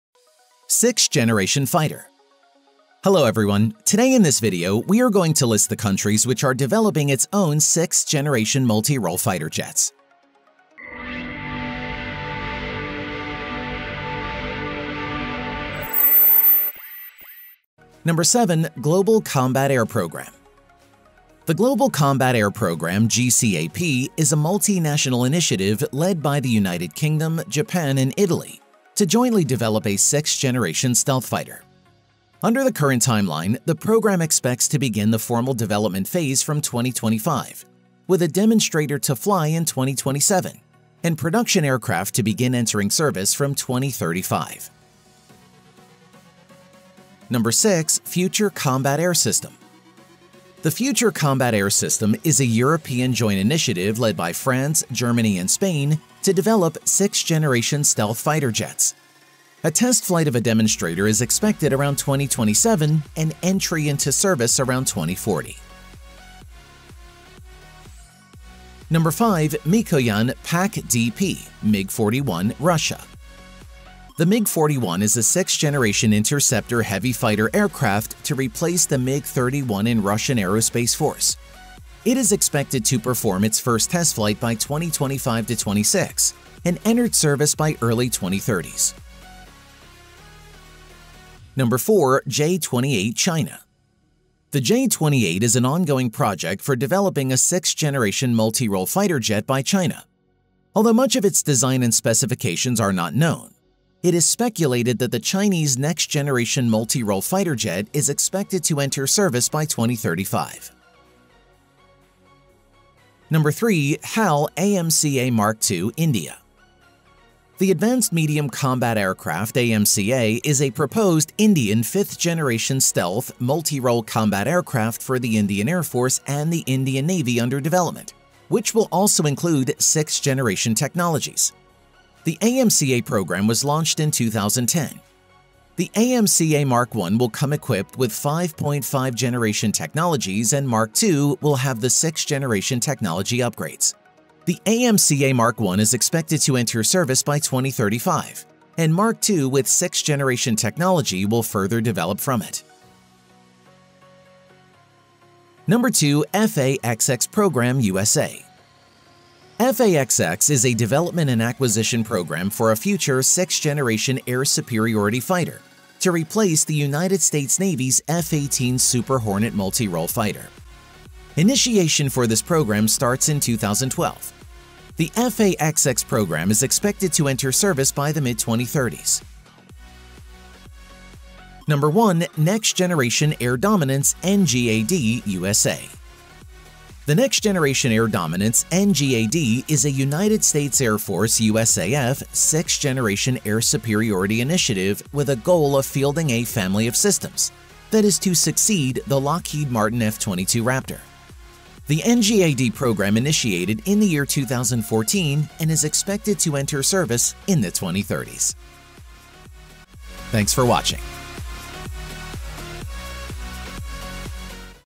Race For Sixth Generation Fighter Jets! Who Will develop It First?
Race For Sixth Generation Fighter Jets! Who Will develop First?
All countries Developing 6th generation fighter jets.

Number 7: Global Combat Air Programme
The Global Combat Air Programme (GCAP) is a multinational initiative led by the United Kingdom, Japan, and Italy to jointly develop a sixth-generation stealth fighter. Under the current timeline, the programme expects to begin the formal development phase from 2025, with a demonstrator to fly in 2027, and production aircraft to begin entering service from 2035.

Number 6: Future Combat Air System

The Future Combat Air System is a European joint initiative led by France,Germany and Spain to develop 6th generation stealth fighter jets.A test flight of a demonstrator is expected around 2027 and entry into service around 2040.

Number 5: Mikoyan PAK DP / Mig 41 -Russia

The Mig 41 is a 6th generation interceptor/heavy fighter aircraft to replace the Mig 31 in Russian aerospace force. It is expected to perform its first flight test by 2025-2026 and entered service by early 2030s.

Number 4:J-28 -China
The J-28 is a ongoing project for developing a 6th generation multirole fighter jets by China.Although much of of its design and specifications are not known.It is speculated that the Chinese next generation multirole fighter jet is expected to enter service by 2035.

Number 3: HAL AMCA MARK 2-INDIA
The Advanced Medium Combat Aircraft (AMCA) is a proposed Indian fifth-generation stealth, multirole combat aircraft for the Indian Air Force and the Indian Navy under development which will also include sixth-generation technologies.The AMCA programme was launched in 2010.
The AMCA Mark 1 will come equipped with 5.5 generation technologies and Mark 2 will have the  6th generation technology upgrades.
The AMCA mark 1 is expected to enter service by 2035 and mark 2 with sixth generation technology will further develop from it.

Number 2: F/A-XX program- USA

F/A-XX is a development and acquisition program for a future sixth-generation air superiority fighter to replace the United States Navy's F-18 super hornet multirole fighter. Initiation for this program starts in 2012. The F/A-XX programme expected to enter service by mid 2030s.

Number 1: Next Generation Air Dominance (NGAD) - USA
The Next Generation Air Dominance (NGAD) is a United States Air Force (USAF) sixth-generation air superiority initiative with a goal of fielding a "family of systems" that is to succeed the Lockheed Martin F-22 Raptor.The NGAD programme initiated in the year 2014 and is expected to enter service in the 2030s.